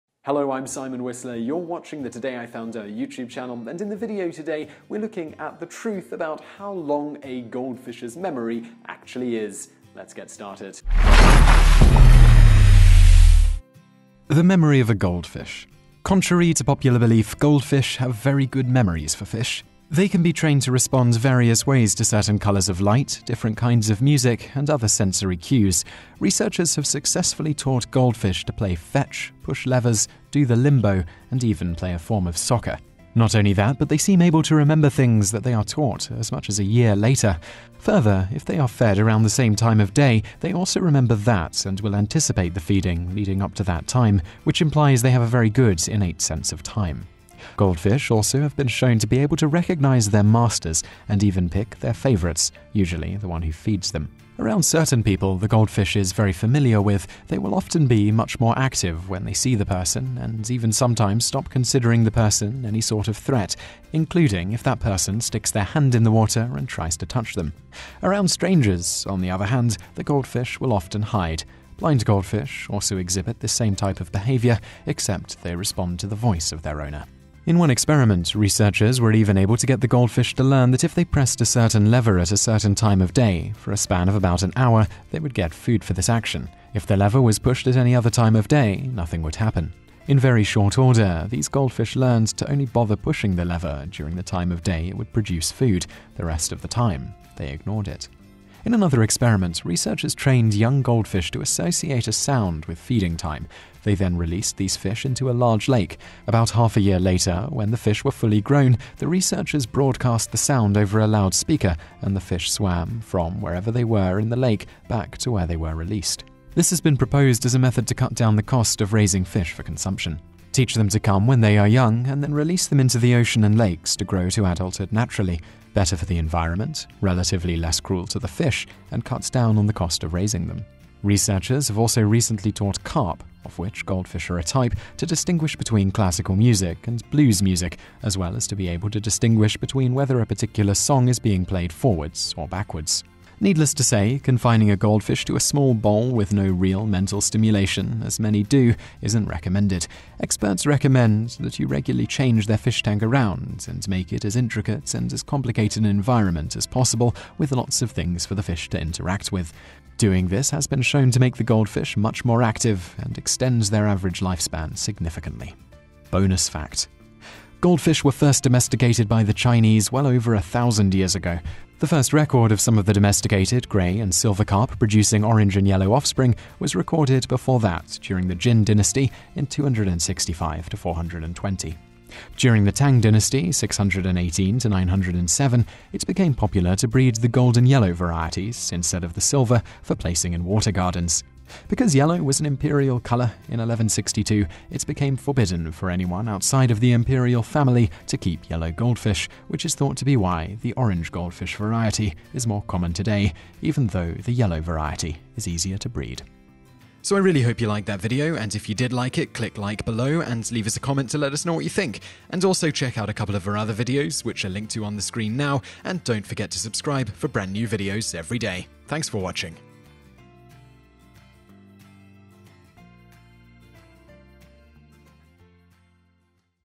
Why You Should Never Leave a Goldfish in a Small Fish Bowl Alone
In this video:

Goldfish actually have very good memories for fish. They can be trained to respond various ways to certain colors of light; different kinds of music; and other sensory cues. Not only that, but they seem to be able to remember things they are taught as much as a year later. Researchers have successfully taught goldfish to play fetch, push levers, do the limbo, and even play soccer. If they are fed around the same time of day, they also remember that and will anticipate the feeding leading up to that time, which implies they have a very good sense of time.